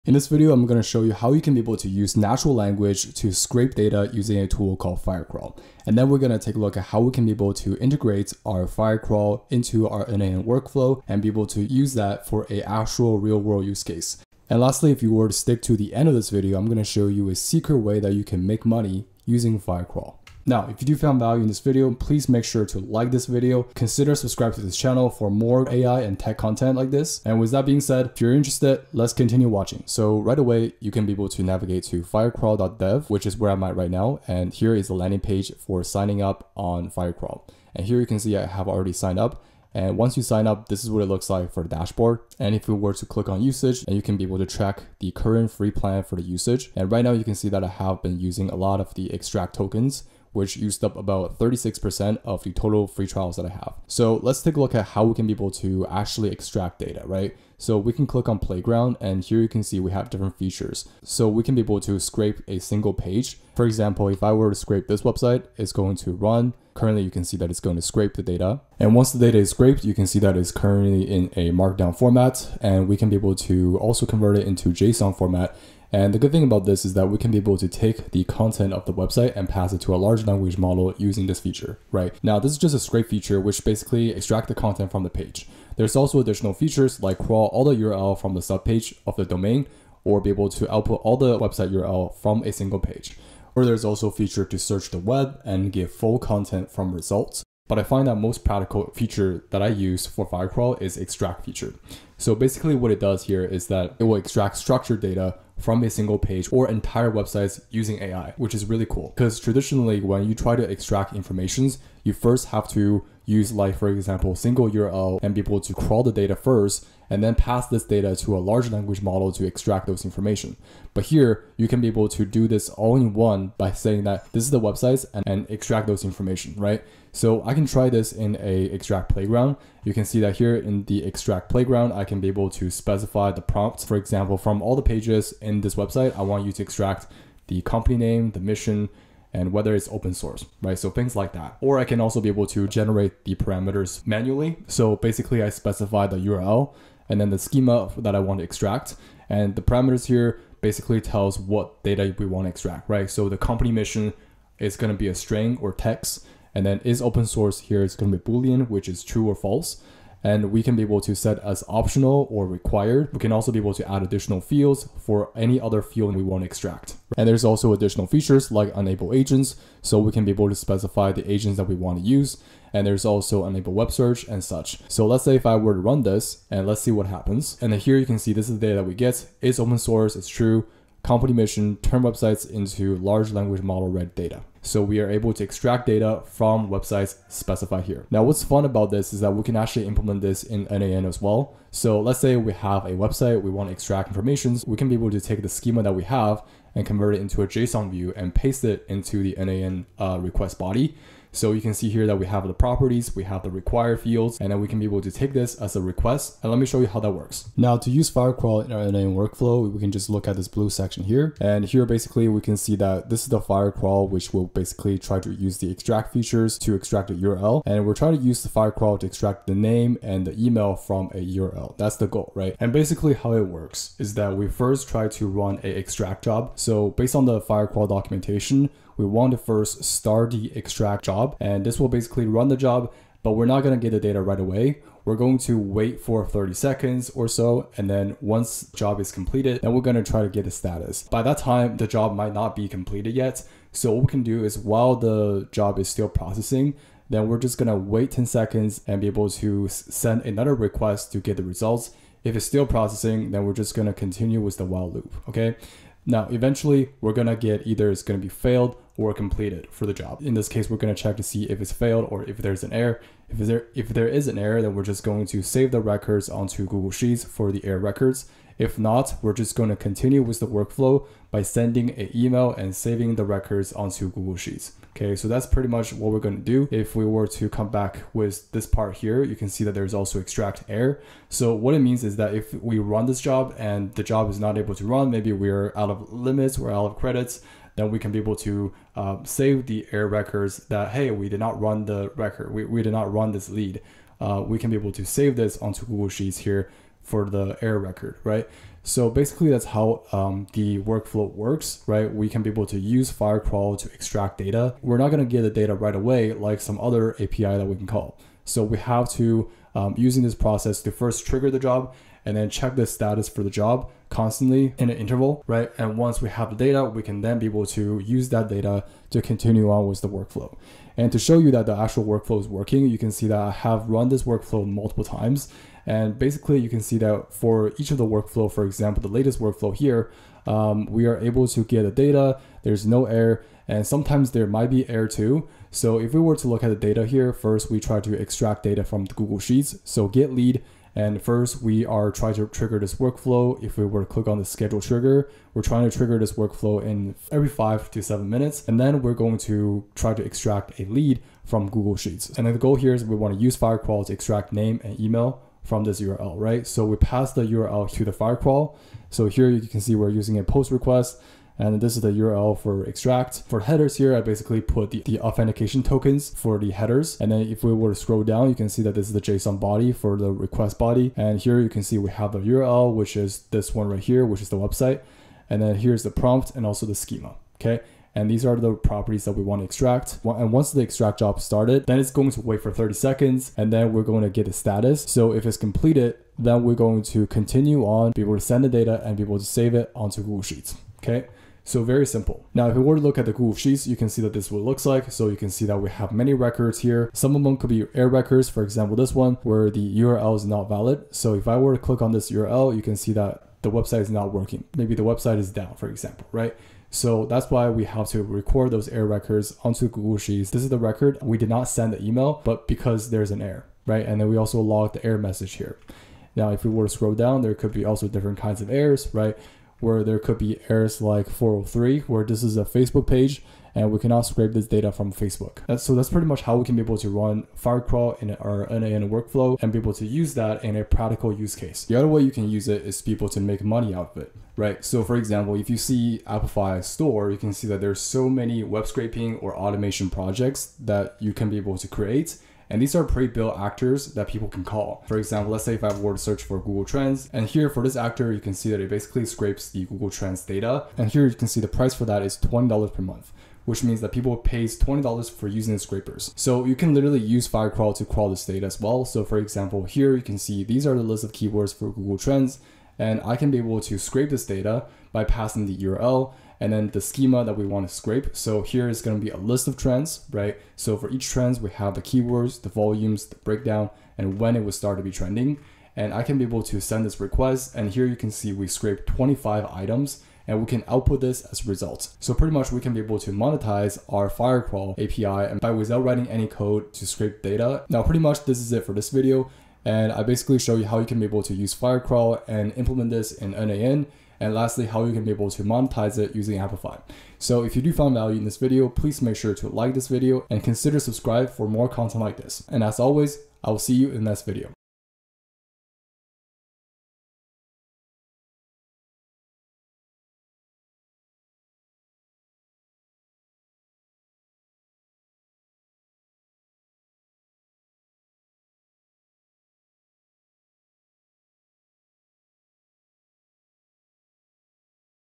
Turn AI Web Scraping into Profit (My Firecrawl & n8n System)
Ready to turn AI web scraping into a profitable system? This video shows you exactly how.

Learn how to use Firecrawl to scrape data from any website using simple natural language. We then build a complete, real-world n8n automation workflow to handle the entire process, including saving successes and failures to Google Sheets. Finally, I'll reveal a practical method to monetize this skill. This is the complete guide.
🔗 RESOURCES & LINKS

TIMESTAMPS:
0:00 - The AI Scraping Method That Profits
0:30 - Getting Started with Firecrawl
1:41 - Extracting Website Data with AI
3:27 - Building the Automated n8n Workflow
7:04 - The Secret to Monetizing This Skill
8:08 - Your Next Steps